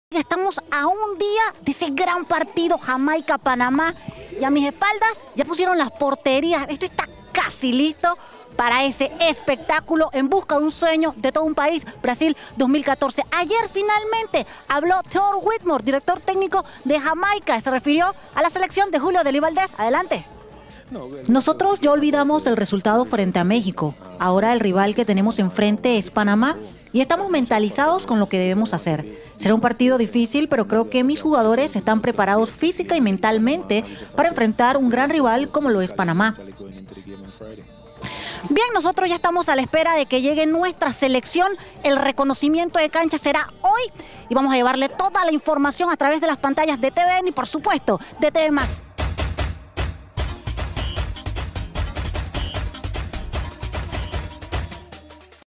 Declaraciones de Theodore Withmore
El técnico de la selección de Jamaica le tiene respeto a Panamá y señaló oque dejó atrás el empate a 0 en el Azteca